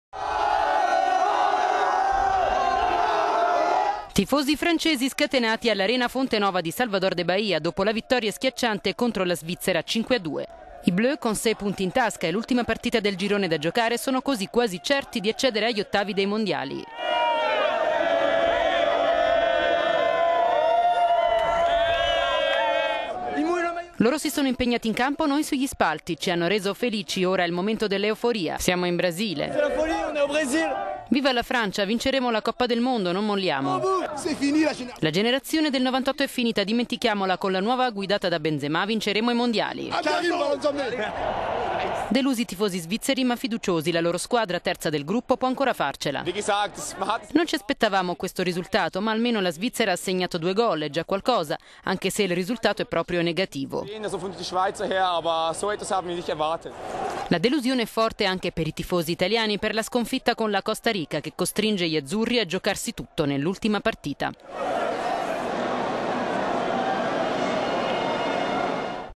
Mondiali: a Salvador de Bahia l'euforia dei tifosi francesi
Tifosi francesi scatenati all' Arena Fonte Nova di Salvador de Bahia dopo la vittoria schiacciante contro la Svizzera, 5-2. I "bleus" con sei punti in tasca e l'ultima partita del girone da giocare, sono così quasi certi di accedere agli ottavi dei Mondiali.

"Loro si sono impegnati in campo, noi sugli spalti. Ci hanno reso felici, ora è il momento dell'euforia. Siamo in Brasile, viva la Francia, vinceremo la Coppa del Mondo, non molliamo".

"La generazione del '98 è finita, dimentichiamola. Con...